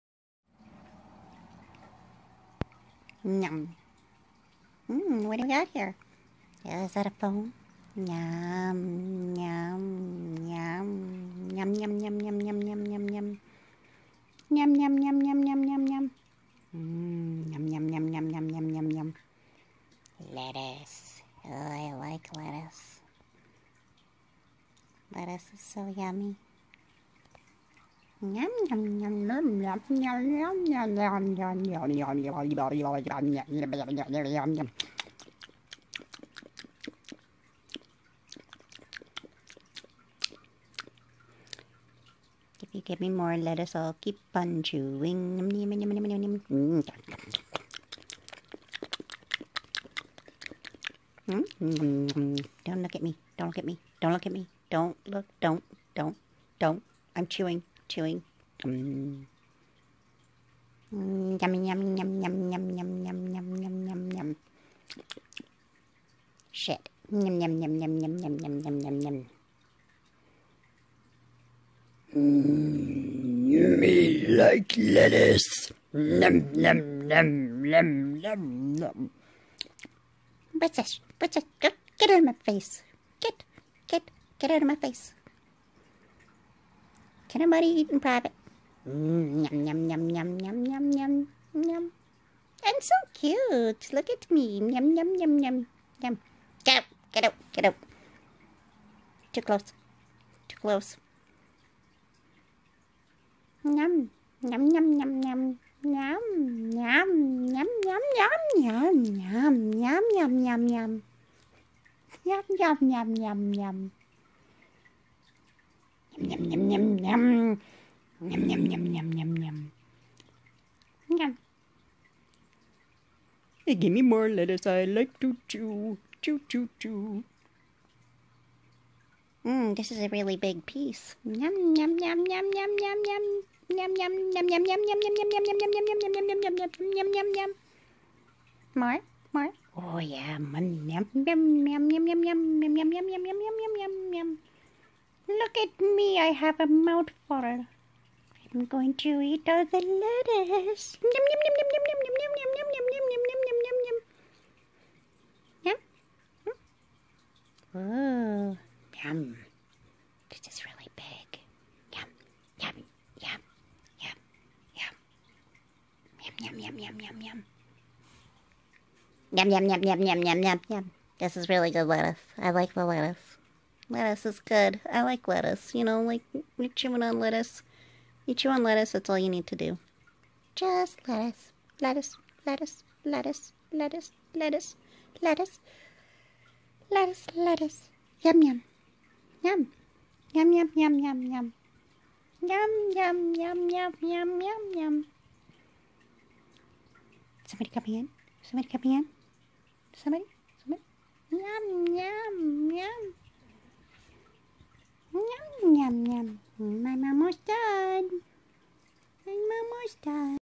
Bunny
Bunny enjoying eating